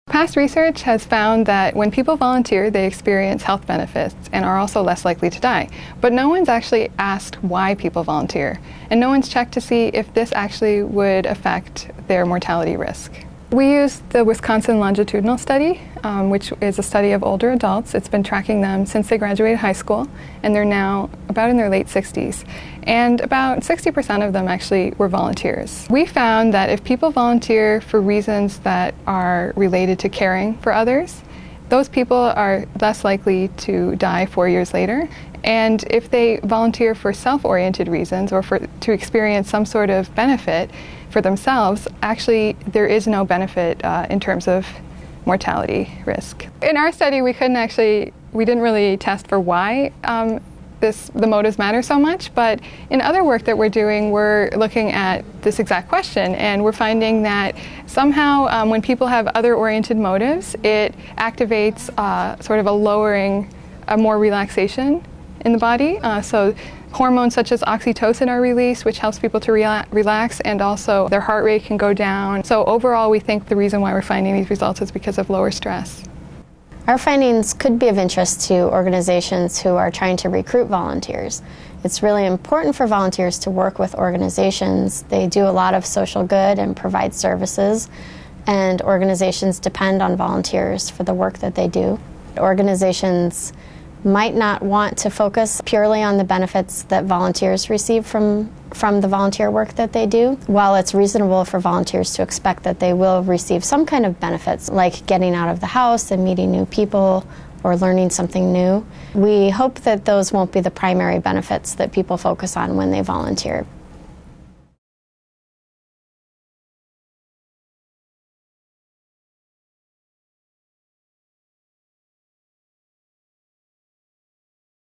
Motives Matter: Why we volunteer affects our longevity
University of Michigan Institute for Social Research social psychologists explain why volunteer motives affect our longevity.